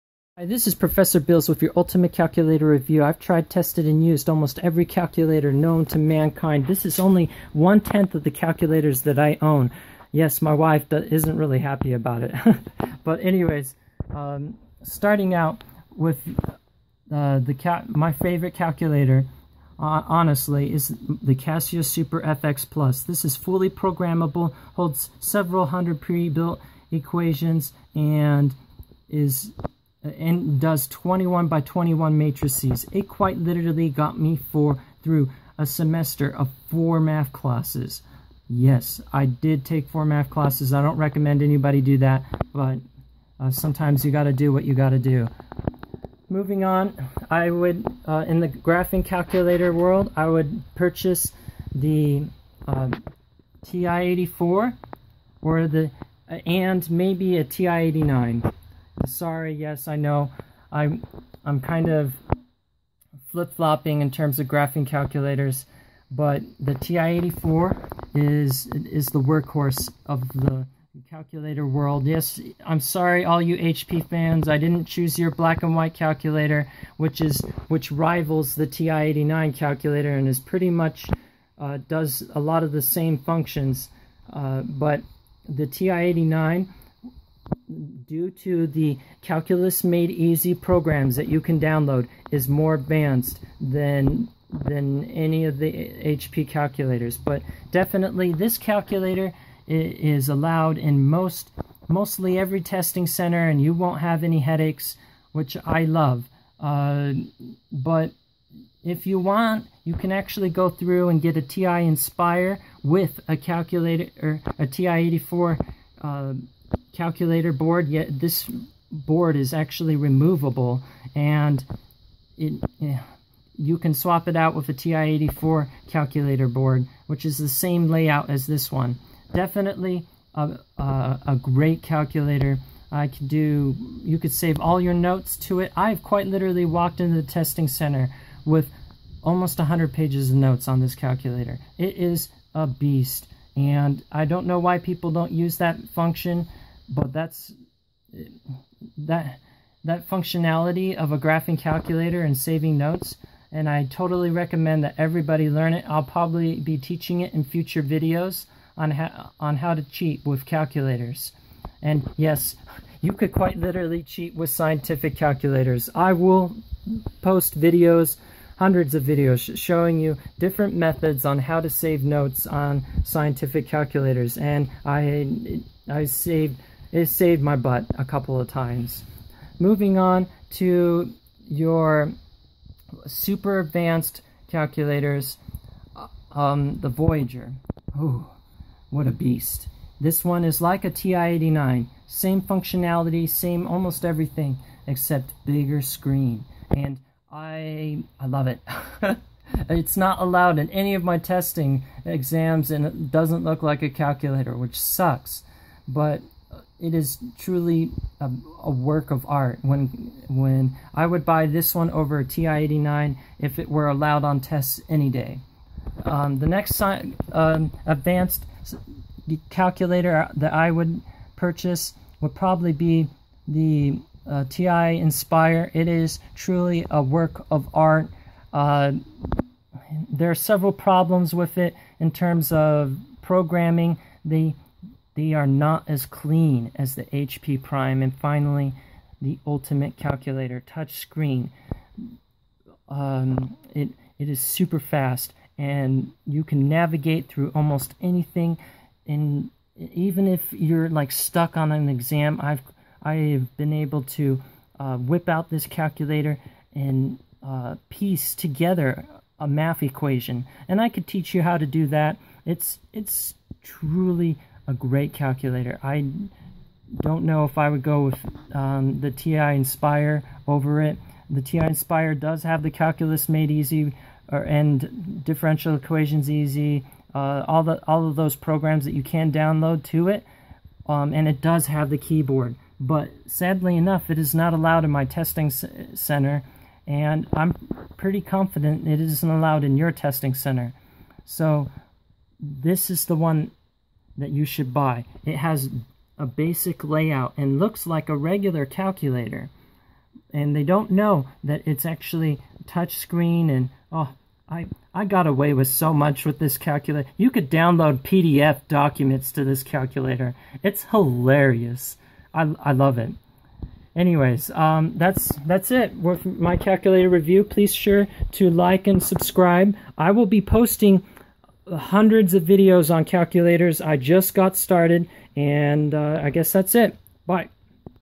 Ultimate Calculator Review!!! Fight Back!! Prof. Bills Casio HP TI Generic
Hi my name is prof. Bills, and I spent 10 years studying how to utilize calculators to their fullest potential.  I have owned and studied them all.  My calculator lot consist of 3,000 calculators.  After school I have taught classes on how to cheat and utilize calculators to their full potential.  I tell everyone that If you teacher gives you a tool, then use it!  Fight back!! Win!!!

If you have any questions I will do my best to answer them.

The calculators that I recommend the most are the:

scientific: Casio Super FX Plus
Calculator-FX-5800P-N/dp/B000NWJI6C

Graphing: TI-84 regular

Advanced: HP prime
HP G8X92AA LA Prime v2 Graphing Calculator